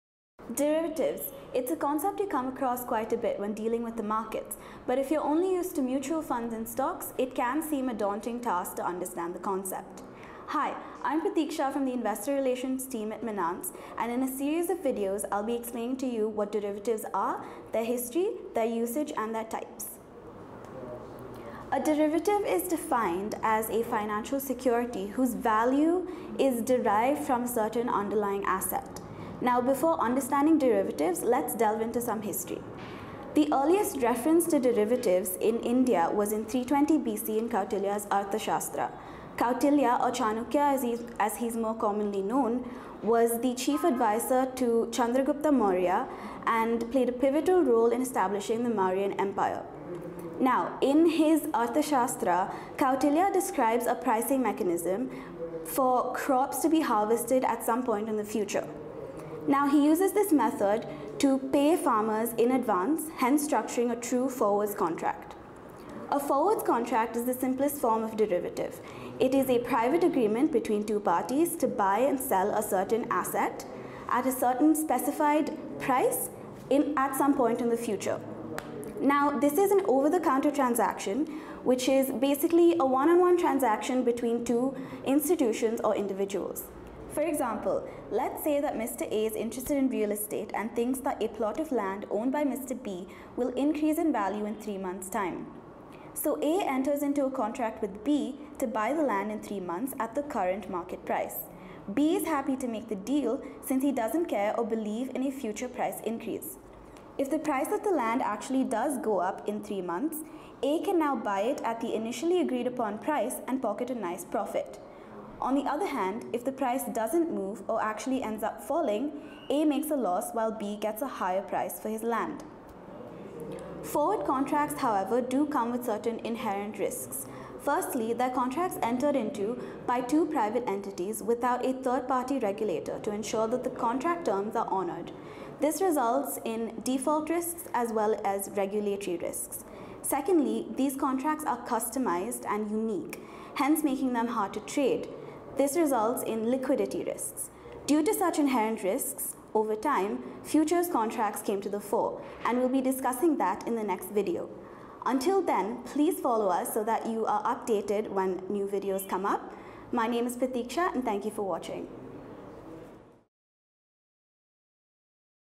Introduction to Derivatives
Derivatives are among the most important financial instruments in modern history. They also have a reputation for being complicated. In a series of videos, we will be addressing that notion.

Minance is a private wealth management firm based out of Bangalore, India. To know more about how we can help you with your investments,